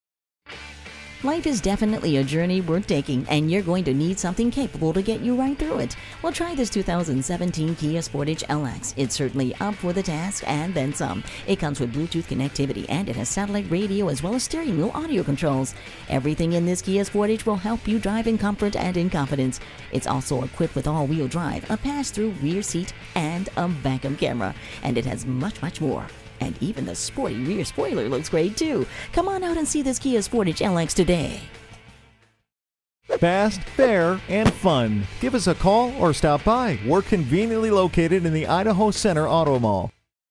2017 Kia Sportage Boise ID Nampa, ID #970007 - SOLD
Year: 2017
Make: Kia
Model: Sportage
Trim: LX AWD
Engine: 4 Cyl - 2.4 L
Transmission: Automatic
Color: Black Cherry
Interior: Black
Stock #: 970007
VIN: KNDPNCAC1H7050306
S How would you like riding away in this fantastic-looking 2017 Kia at a price like this? You just simply can't beat a Kia product. It is nicely equipped.

Address:
15700 Idaho Center Blvd